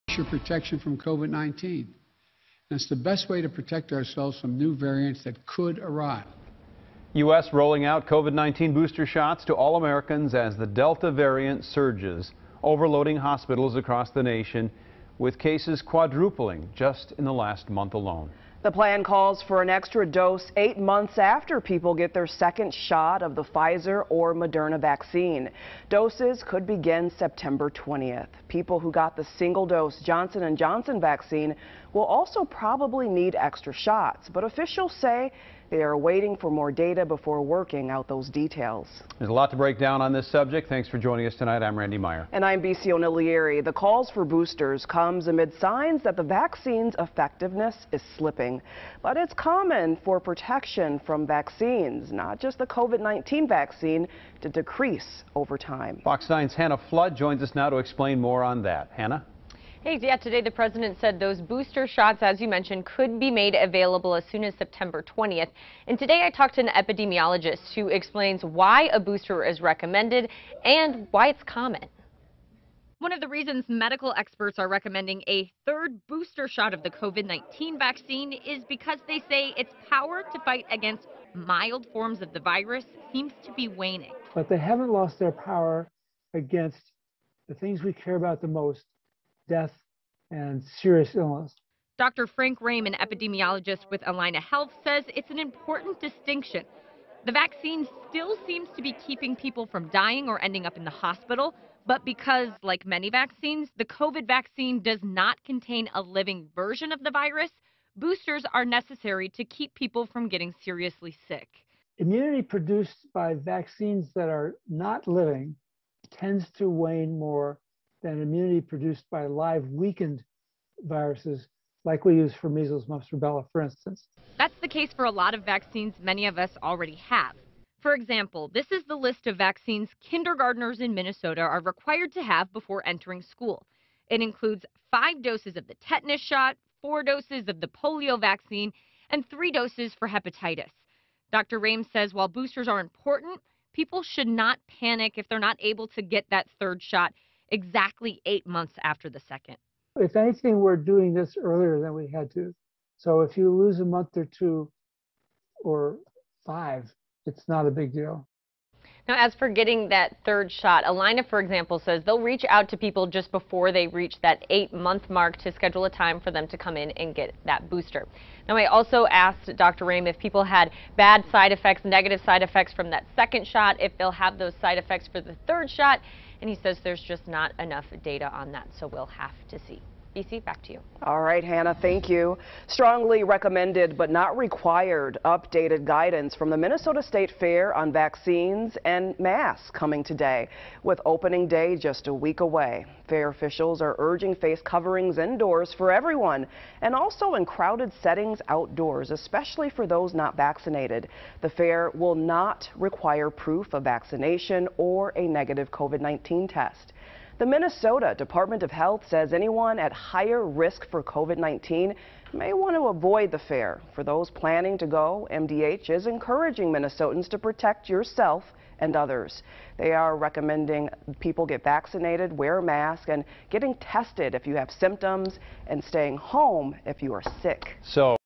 Why some shots, like the COVID-19 vaccine, need boosters | FOX 9 KMSP
Health officials are directing people to receive a COVID-19 booster shot amid signs the vaccine’s effectiveness may wear off over time, which is not uncommon among certain vaccines.